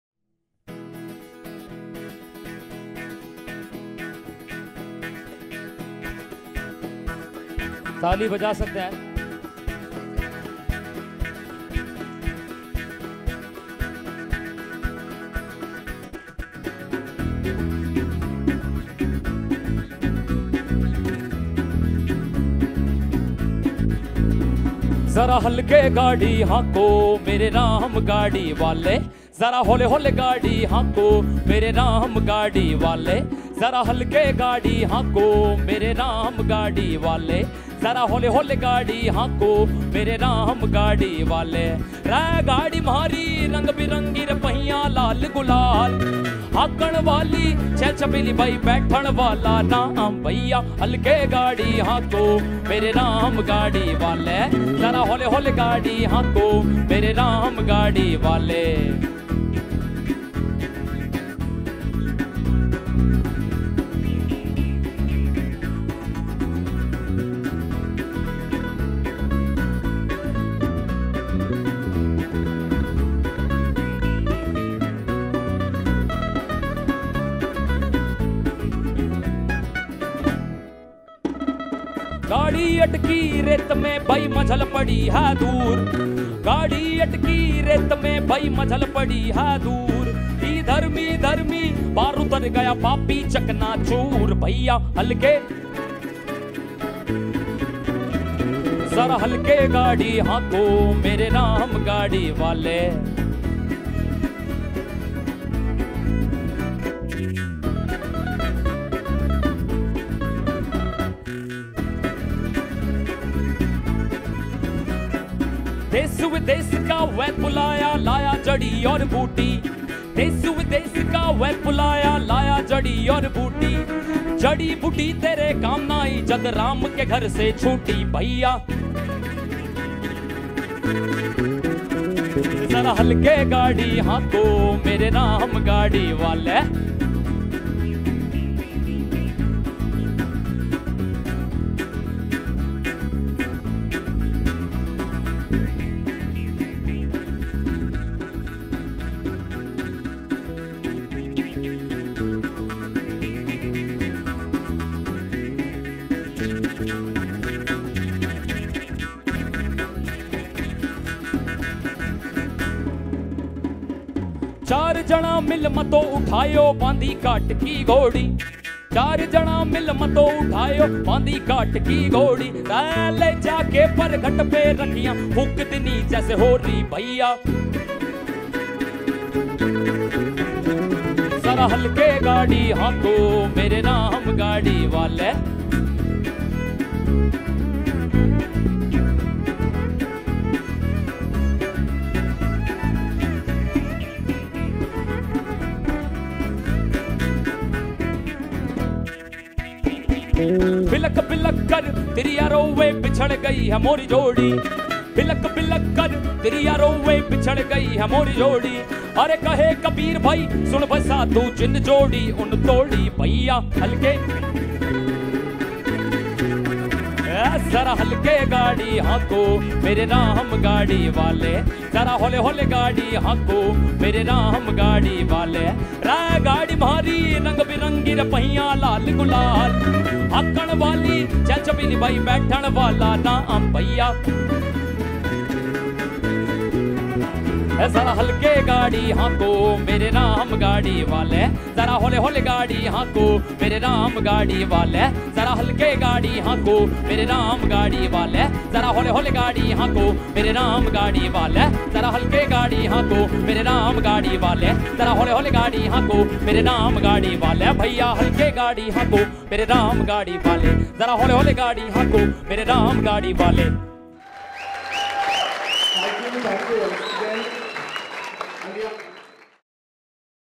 NCPA OWMM Band Baja Contest Winners: Neeraj Arya's Kabir Cafe
Winners of NCPA's One World Many Musics (OWMM) performing at Tata Theatre Foyer